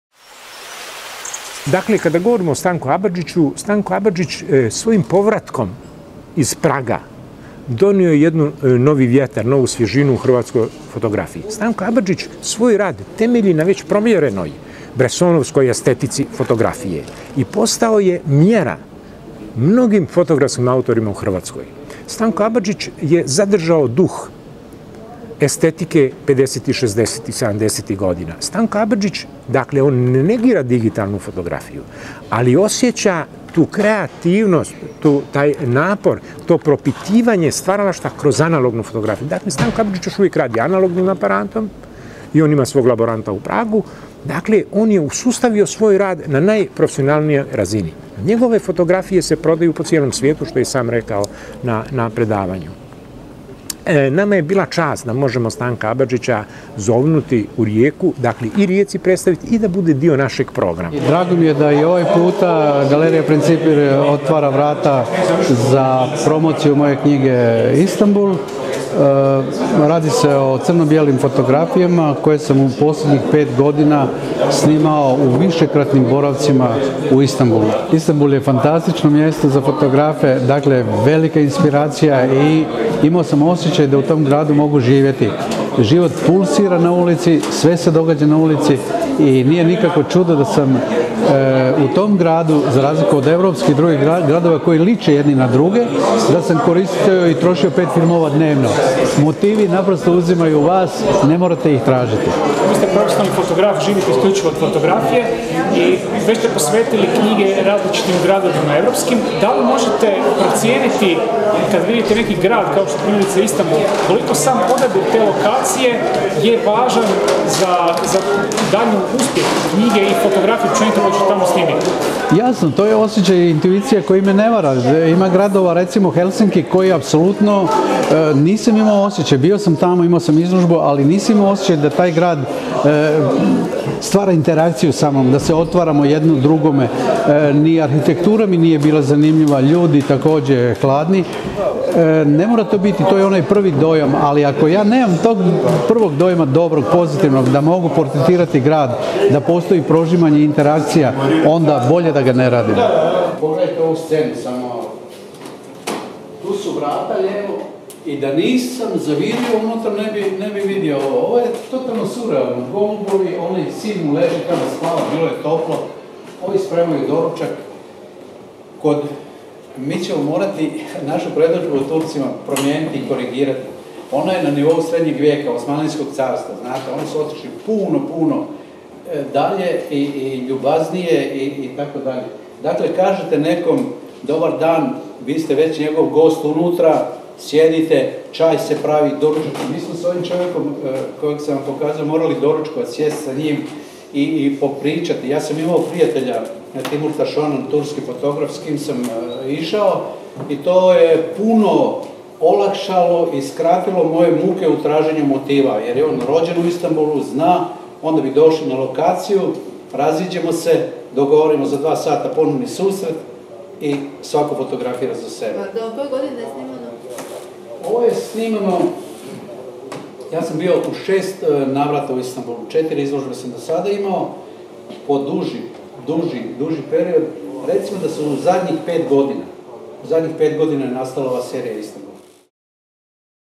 MojaRijeka.hr - Stanko Abadžić "Istanbul"
U sklopu Festivala fotografije u Rijeci "PHOTORI" održano je 26. rujna u prostoru Galerije Principij predavanje i predstavljanje knjige "Istanbul" fotografa Stanka Abadžića.